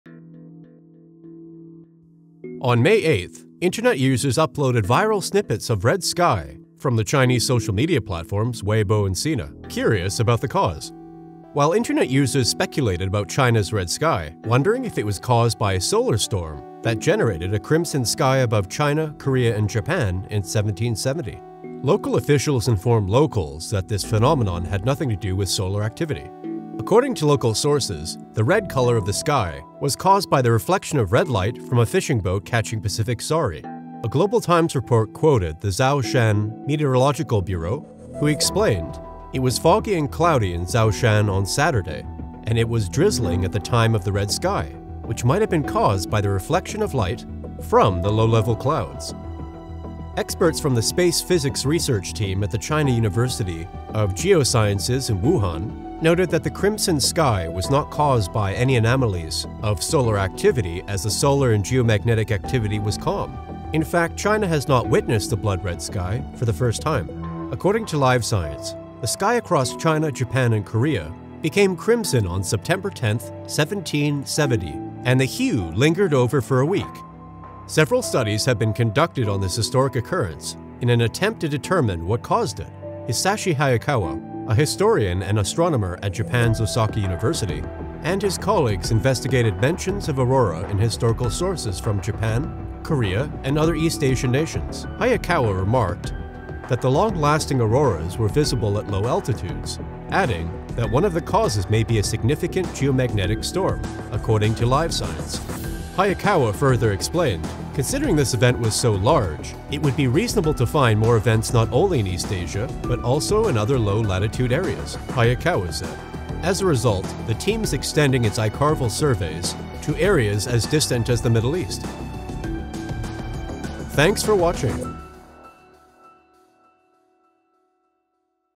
EXPLAINED: WHY CHINA HAD BLOOD RED SKY?
On May 8th, Internet users uploaded viral snippets of the red sky from the Chinese social media platforms Weibo and Sina, curious about the cause.

While internet users speculated about China's red sky, wondering if it was caused by a solar storm that generated a crimson sky above China, Korea, and Japan in 1770, local officials informed locals that this phenomenon had nothing to do with solar activity.
According to local sources, the red colour of the sky was caused by the reflection of red light from a fishing boat catching Pacific saury.
A Global Times report quoted the Zhoushan Meteorological Bureau who explained, “It was foggy and cloudy in Zhoushan on Saturday and it was drizzling at the time of the red sky, which might have been caused by the reflection of light from the low-level clouds.”

Experts from the space physics research team of the China University of Geosciences in Wuhan noted that the crimson sky was not caused by any anomalies of solar activity as the solar and geomagnetic activity was calm.

In fact, China has not witnessed a blood-red sky for the first time. According to LiveScience, the sky across China, Japan, and Korea became crimson on September 10, 1770, and the hue lingered for over a week.

Several studies have been conducted on this historic occurrence in an attempt to determine what caused it. Hisashi Hayakawa, a historian and astronomer at Japan's Osaka University, and his colleagues investigated mentions of aurora in historical sources from Japan, Korea, and other East Asian nations.
Hayakawa remarked that the long-lasting auroras were visible at low altitudes, adding that one of the causes may be a significant geomagnetic storm, according to LiveScience.
Hayakawa further explained, “Considering this event was so large, it would be reasonable to find more events not only in East Asia but also in other low-latitude areas,” Hayakawa said.  As a result, the team is extending its archival surveys to areas as distant as the Middle East.”

Narrated by:
Max Culina